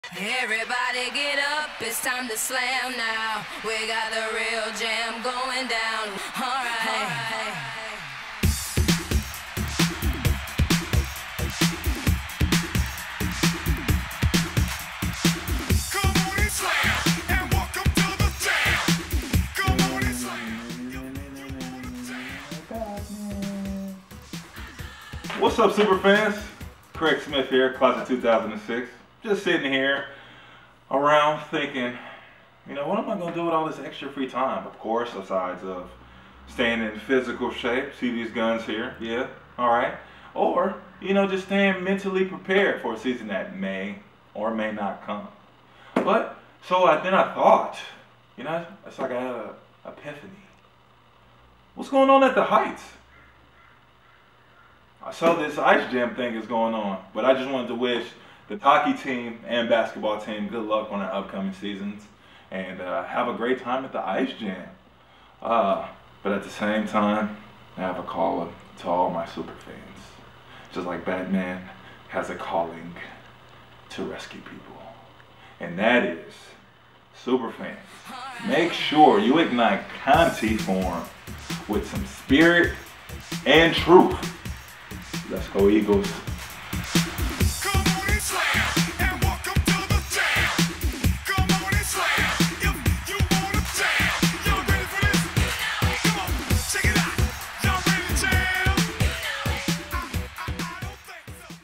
Craig Smith is Ready for Ice Jam 2011! Are you?
Remember Super Fans, Ice Jam is tomorrow night, Thursday October 27 at 7:30 pm!

Hosted by ESPN's STEVE LEVY!  There will be CASH PRIZES, T-SHIRT GIVE AWAYS, CONTETS, MUSIC, AND SO MUCH MORE!  DON'T MISS THIS EVENT!

What is Ice Jam you ask?  Conte Forum and Kelley Rink Meet to form a half ice, half court monster perfect for a Boston College JAM like you've never seen before!  Be there and don't miss any of the exciting action!  See below for more details!

What: Boston College's 2nd annual Ice Jam featuring half court and half ice.
When: Thursday, October 27th at 7:30 pm
Who: Featuring Men's and Women's Basketball and Hockey Teams, Host Steve Levy, and other special guests!

Details:

Ice Jam is FREE and open to the public
First 1500 through the door get a free t shirt along with other great giveaways.
Be in Conte Forum at 7:30 for your chance to win free tuition!
$1000 cash prize for best Halloween costume

Don't miss your chance to learn more about your BC athletes and participate in fun contests!

Students: Want the chance to participate in a 3 point contest against basketball players at Ice Jam?